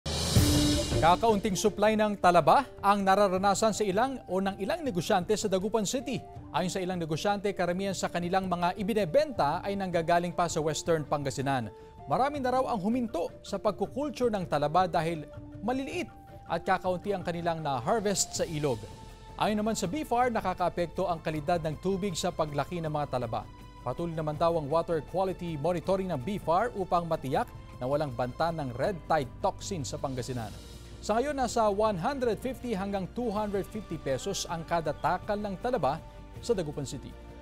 One North Central Luzon: Mababang suplay ng talaba, nararanasan ng ilang negosyante sa Dagupan City
Mababang suplay ng talaba, nararanasan ng ilang negosyante sa Dagupan City.